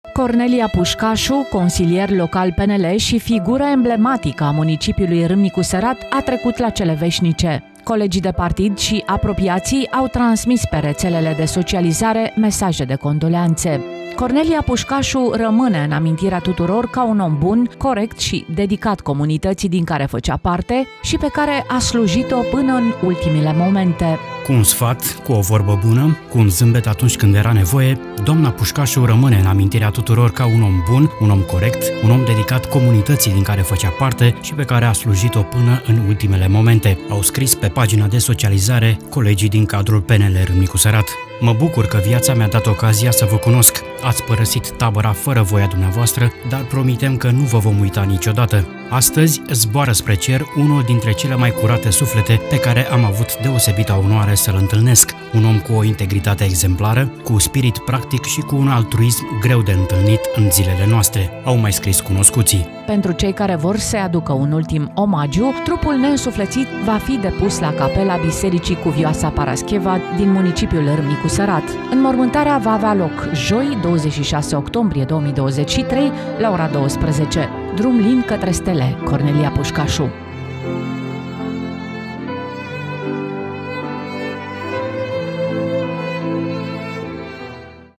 S A STINS DIN VIAȚĂ O FIGURĂ EMBLEMATICĂ RÂMNICULUI
Cornelia Pușcașu, consilier local PNL și figură emblematică a municipiului Râmnicu Sărat, a trecut la cele veșnice